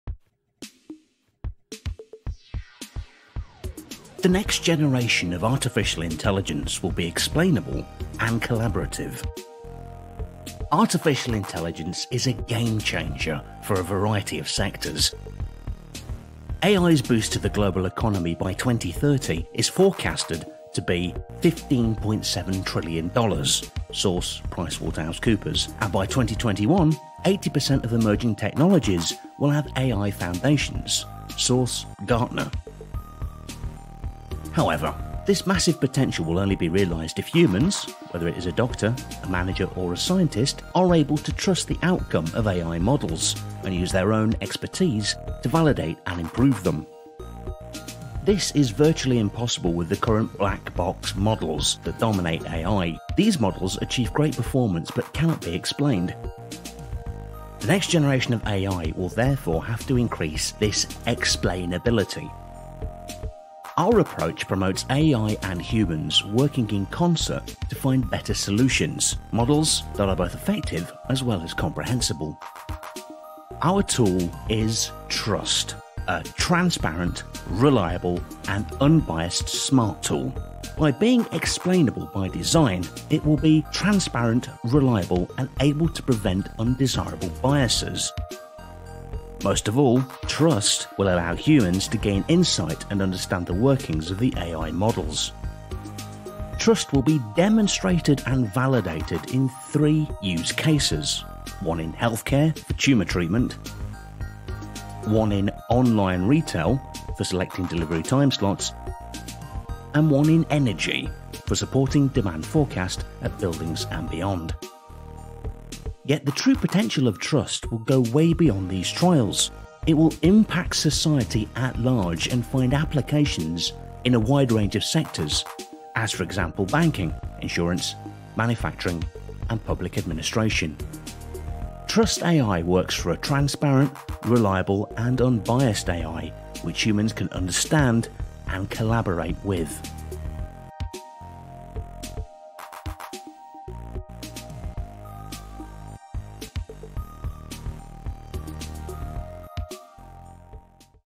Interpretable AI: An introduction to TRUST AI
A 2.5 min introduction to the TRUST-AI project HORIZON (FET) project. The project will last till the end of September 2023.